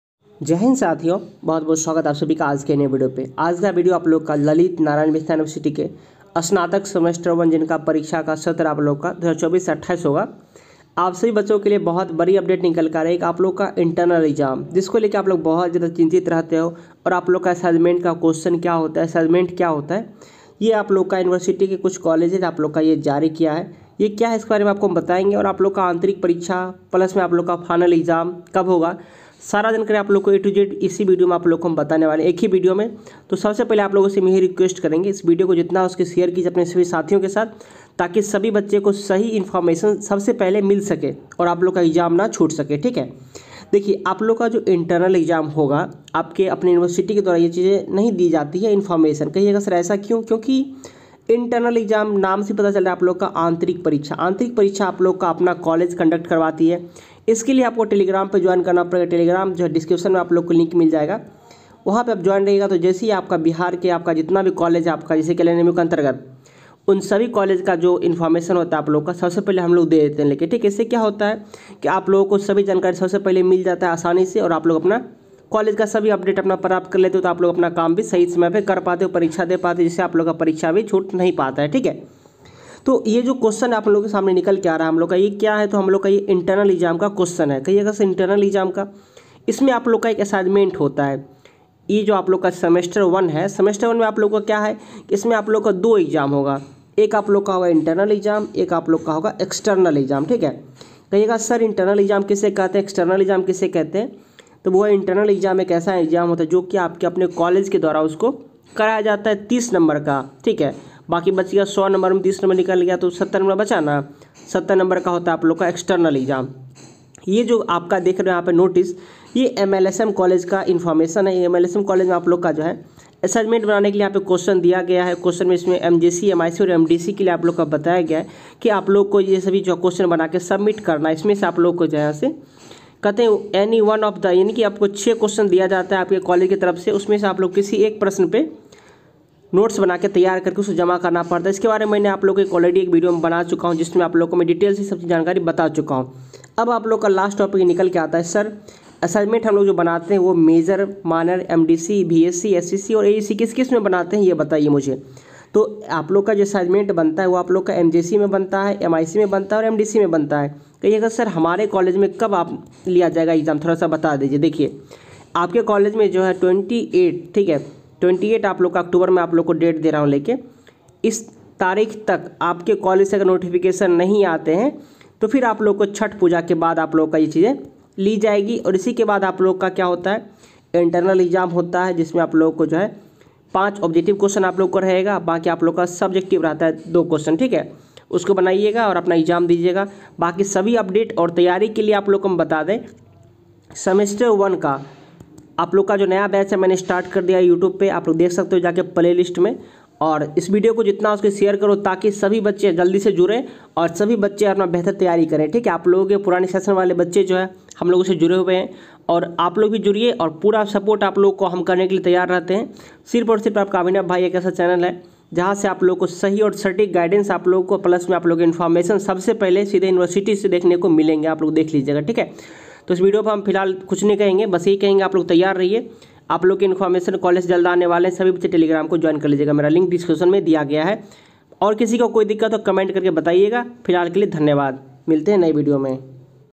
LNMU UG 1st Semester Exam 2024 | LNMU UG 1st Semester Internal Exam Date सत्र 2024-28 इस दिन से ✅
इस वीडियो के माध्यम से ललित नारायण मिथिला विश्वविद्यालय के अंतर्गत स्नातक सेमेस्टर -1 परीक्षा से संबंधित सभी जानकारी को बताया गया है ||